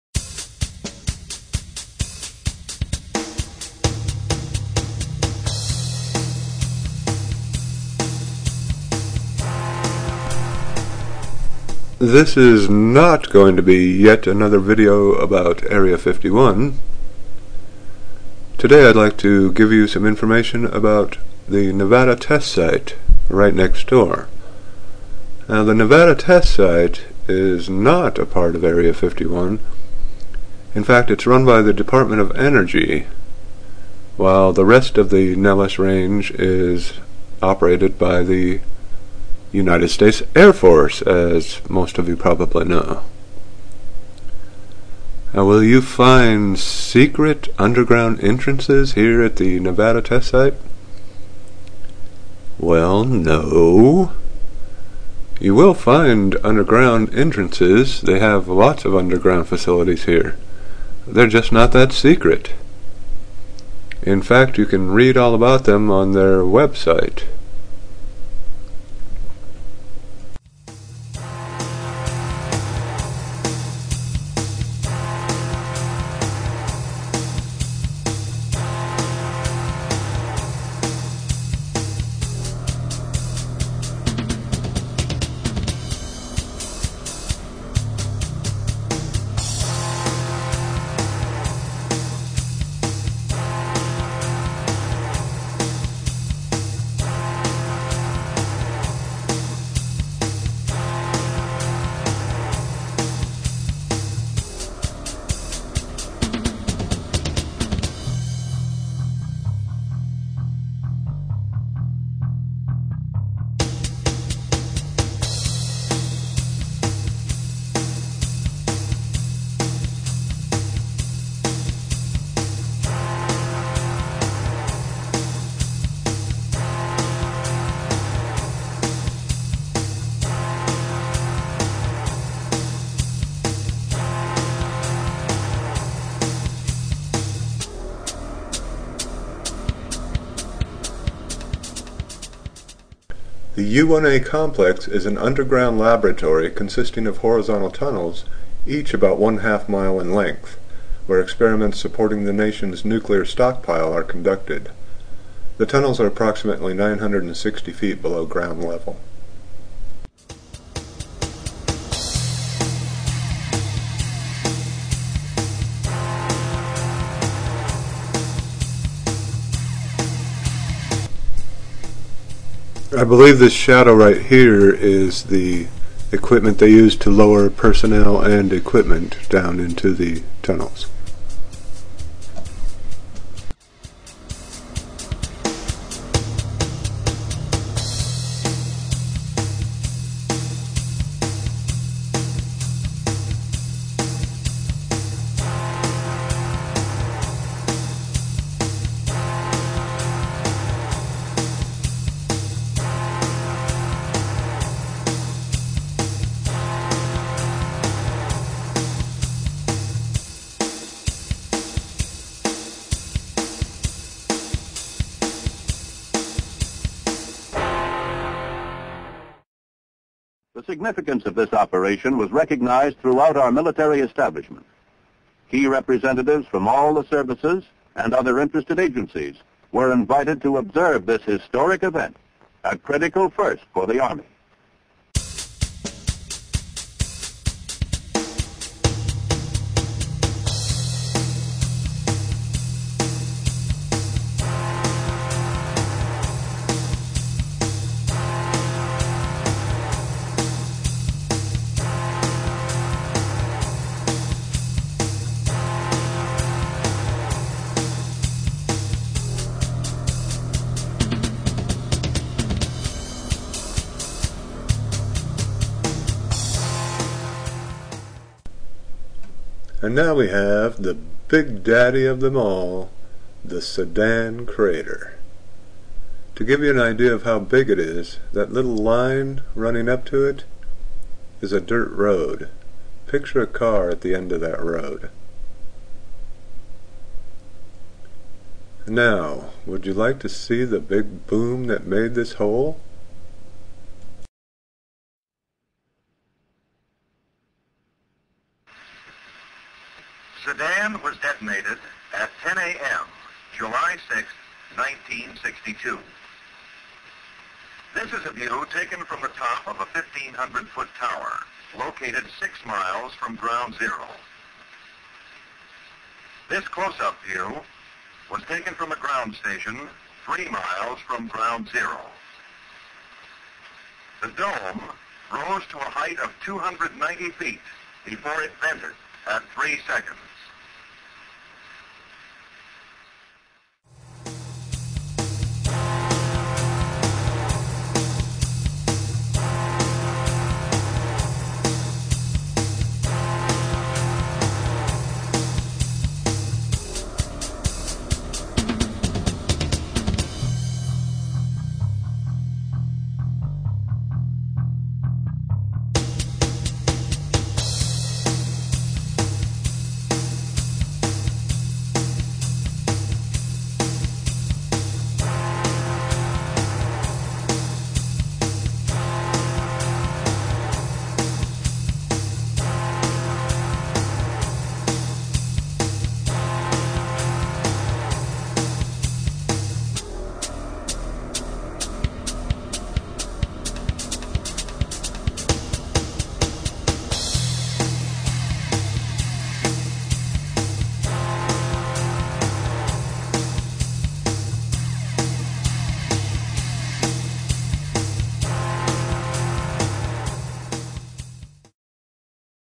Google Earth Tour of Nevada Test Site (with DoE photos and video)
Next door to Area 51, the Nevada Test Site is a restricted area doing classified research BUT... not nearly as secret as some people imagine. The Dept. of Energy actually gives free tours of the area, something the Air Force probably won't be doing with Area 51. ;)

This video includes historic footage of the Sedan explosion that made the world's biggest man-made crater.